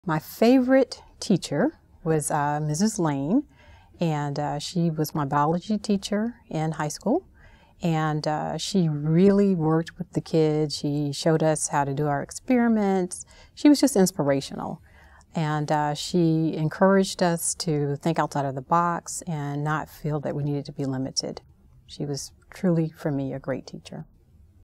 Bioengineer: Helpful Teachers - Vanessa Wyche Career Girls Role Model
Bioengineer Vanessa Wyche shares valuable career guidance and life advice with girls.